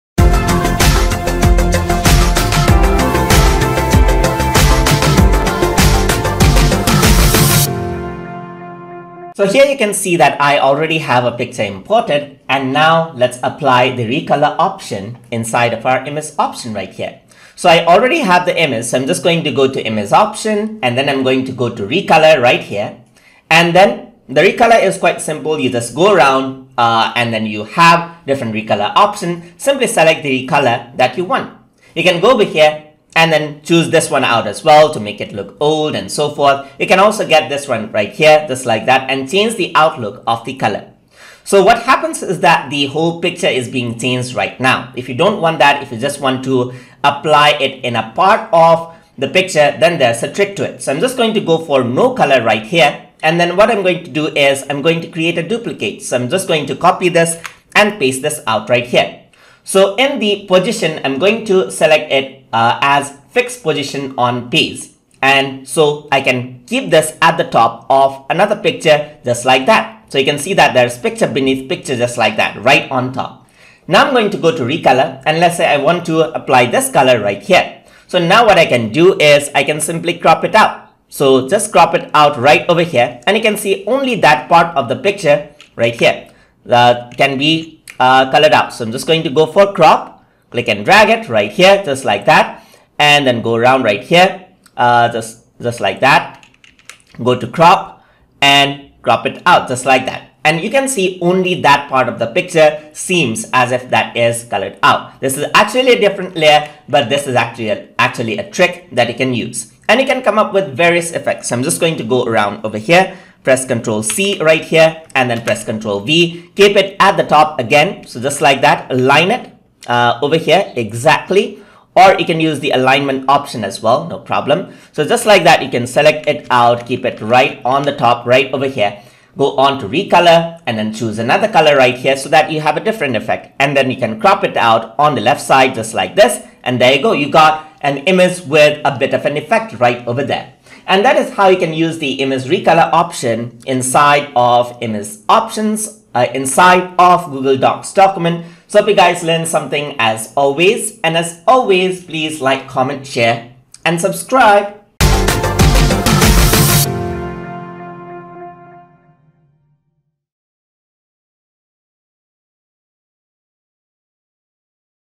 Google Docs Tutorial - Lesson 33 - Image Recolor Options in Image Options
In this tutorial, we will be discussing about Image Recolor Options in Image Options in Google Docs

Learn the basics of using Google Docs including how to write, edit and manage contents.

This lesson has been made using Google Doc using Chrome Browser but is applicable for Mobile App Version of the Application as well.

If you like this video, here's our entire playlist of Google Docs tutorials: